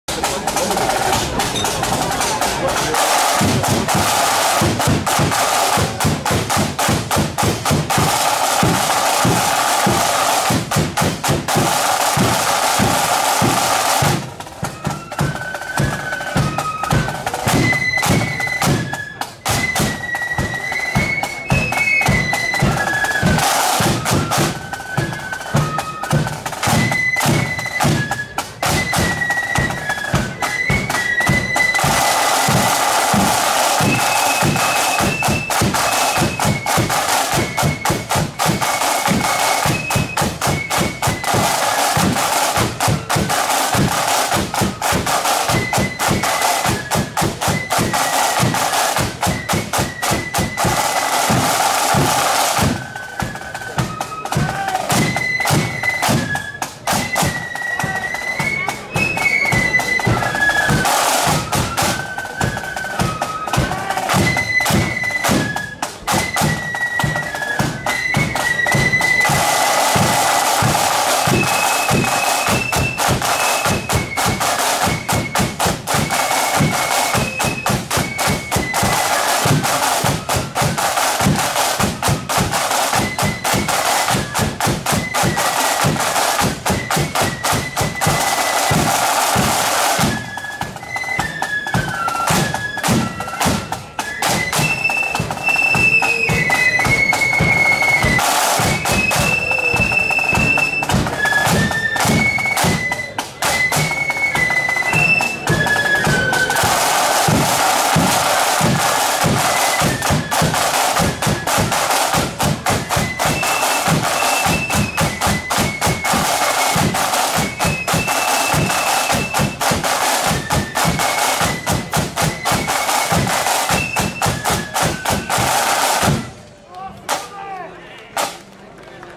Armagh True Blue's (No.19) @ Their Own Parade ~ Armagh ~ 17/08/24 (4K)
Armagh True Blues Flute Band parading through Armagh at the end of the night at their own annual parade which was held in the Cathedral City on Saturday 17/08/2024.